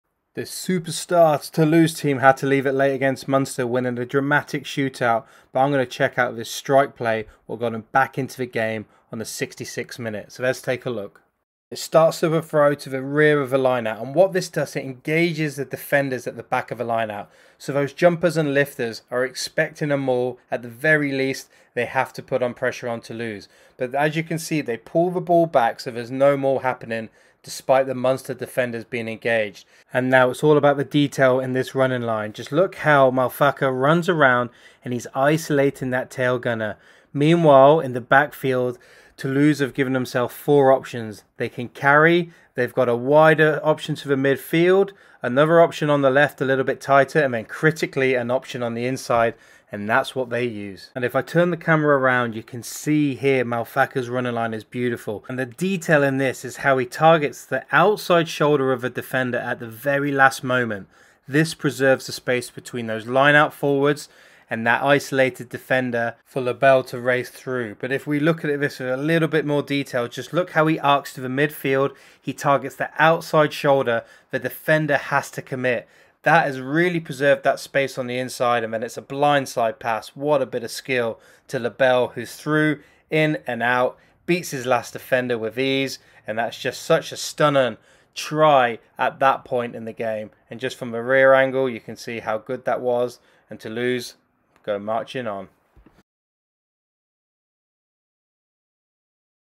Rugby Analysis: Toulouse Strike Play Try (Essai Toulousain)
Rugby Try: Toulouse versus Munster, Champions Cup, Lineup Strike Play